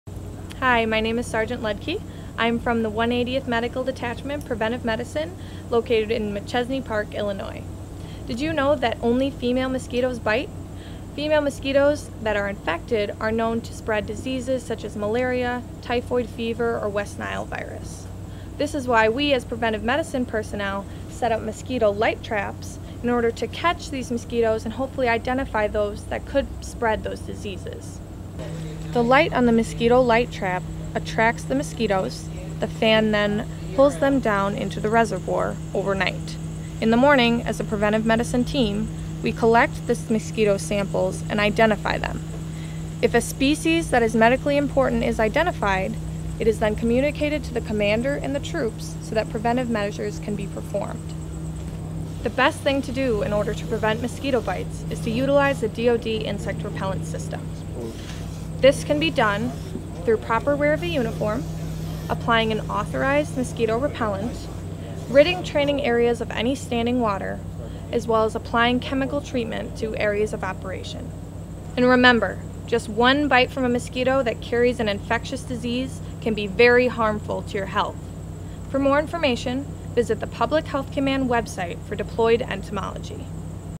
Mosquito public service announcement with Sgt. Melissa Ludke
Sgt. Melissa Ludke of the 180th Medical Detachment Preventive Medicine from Machesney Park, Illinois, speaks about mosquito bites. This is one of the many missions of her U.S. Army Reserve unit during exercises like Global Medic at Fort McCoy, Wisconsin this summer.